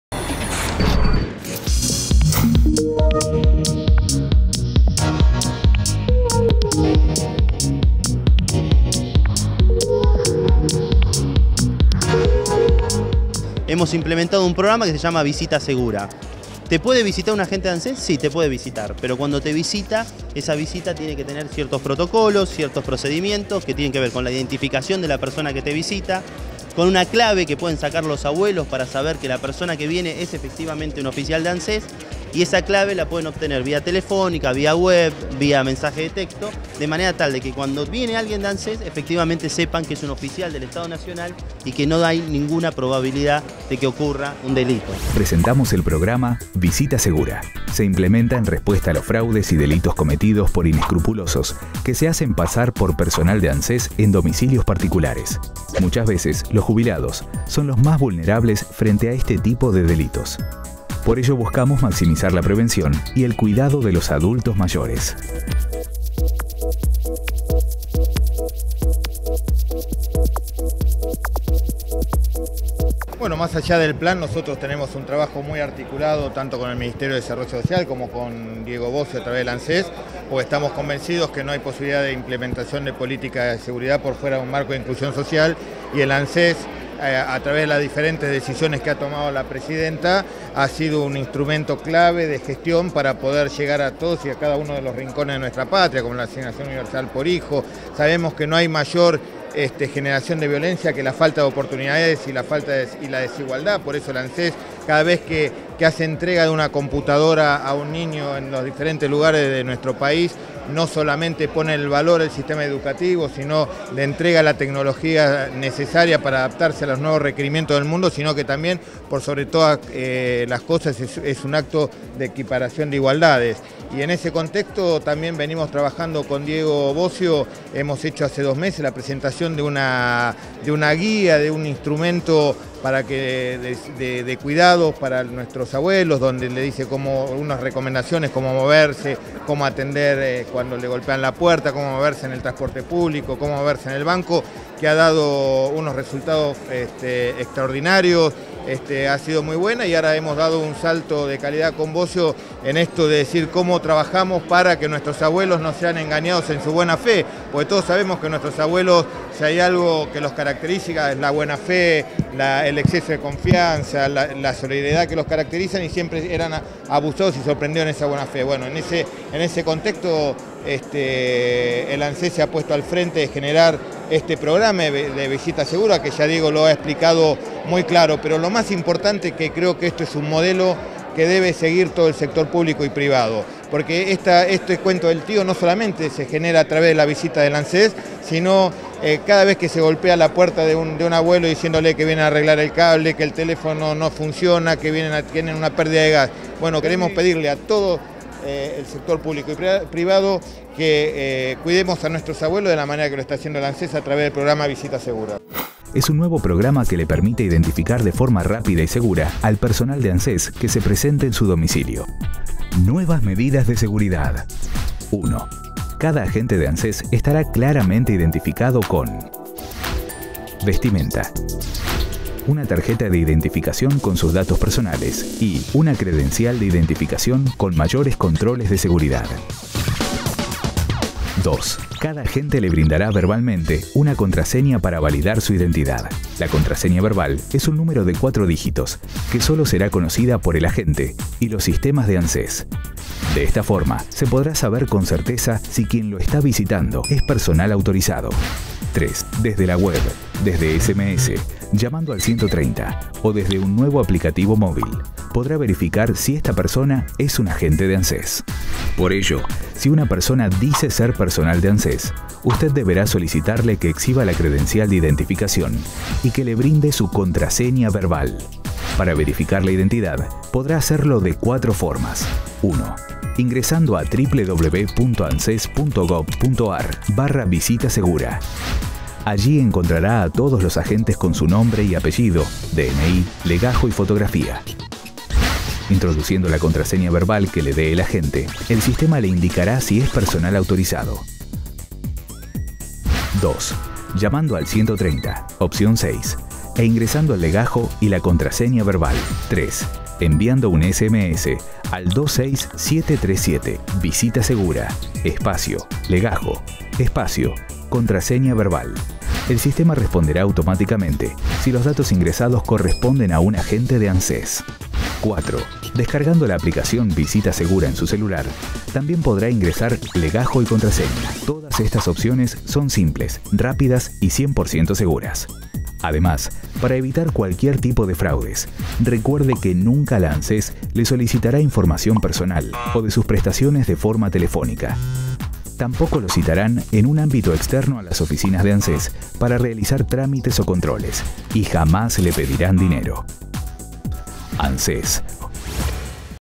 Prevenir TV - 26/10 - Presentación del Programa "Visita Segura"
Estuvieron presentes el Titular del Anses, Diego Bossio y el Secretario de Seguridad, Sergio Berni.